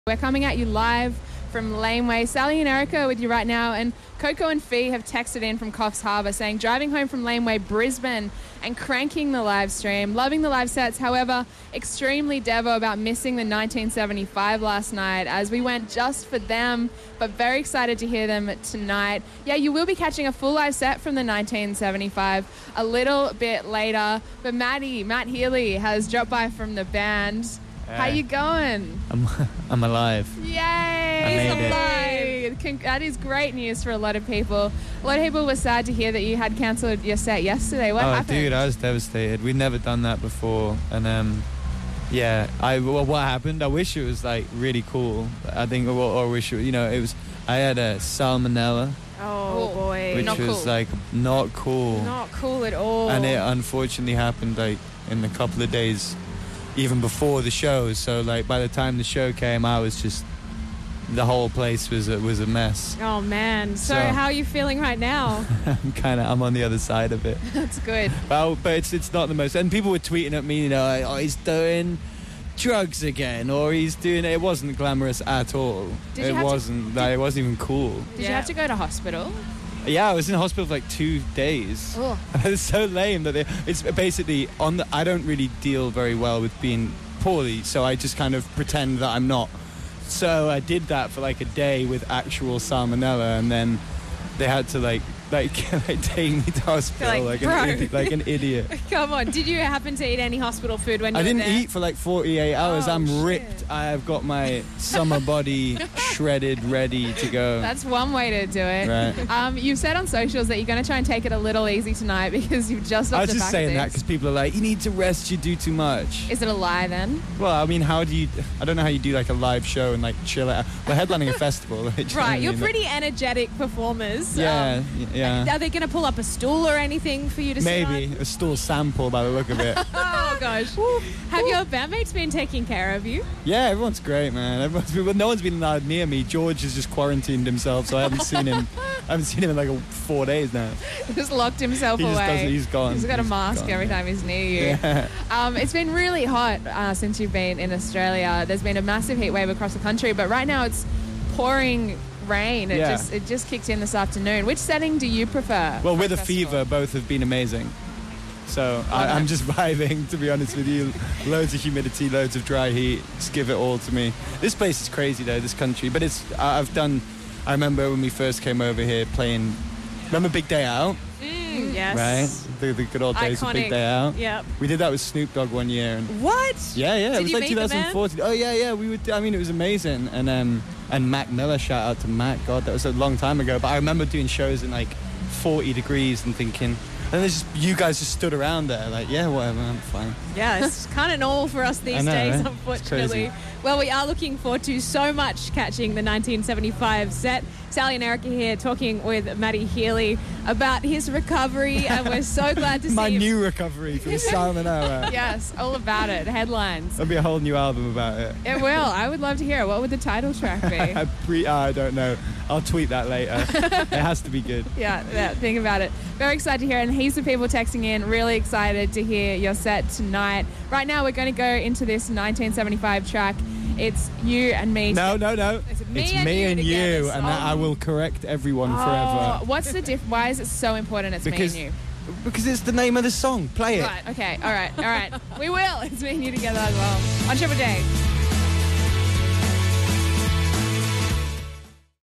The 1975 Matty Healy Interview 2020 - Sydney Laneway Festival - Triple J
2nd February 2020